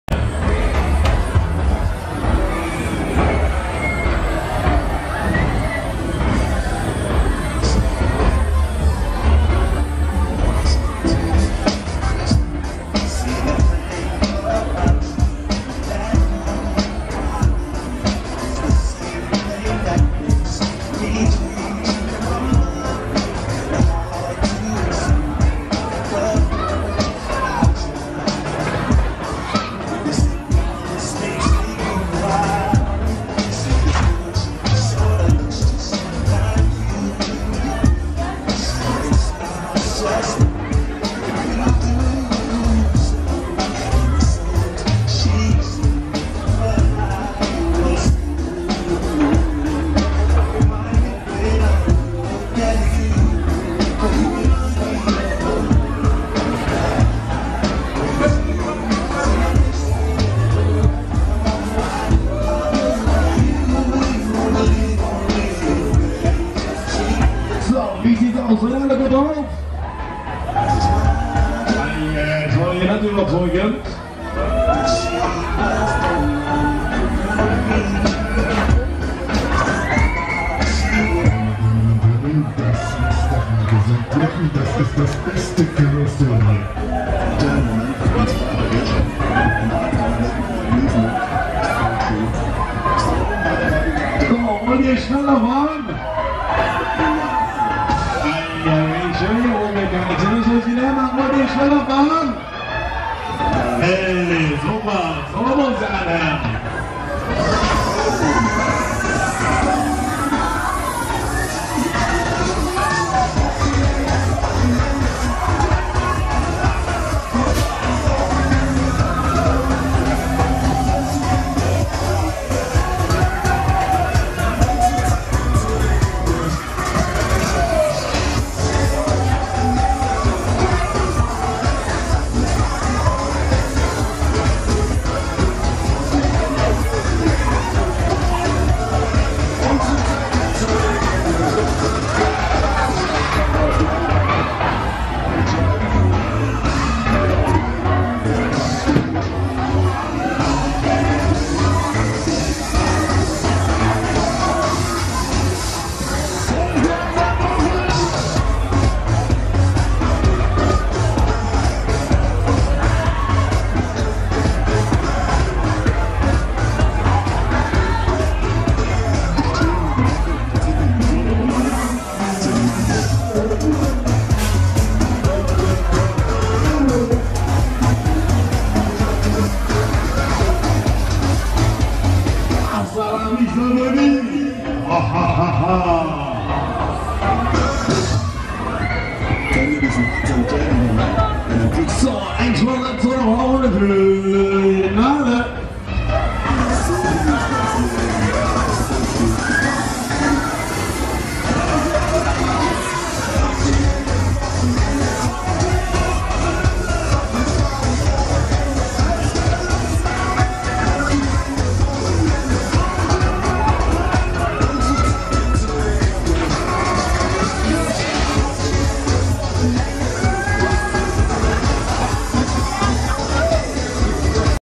Jaguar Express Geisler Offride Video von der Frühjahrs Dippemess 2019 in Frankfurt
( Mack / Berg - und Talbahn ) Jaguar Express Geisler Frankfurter Frühjahrs Dippemess 2019.